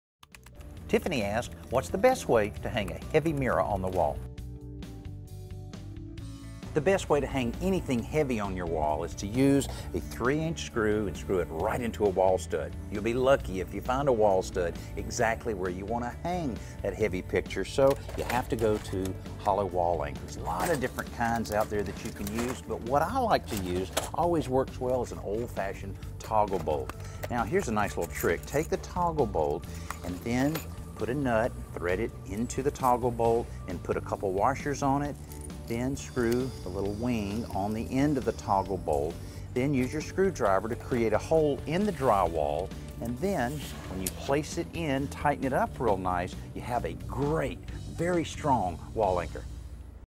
Using a Winged Toggle Nut to Hang Heavy Pictures and Mirrors
Just putting a nail in a wall isn't enough when you're hanging large items, like mirrors and bib pictures. Here's an easy fix.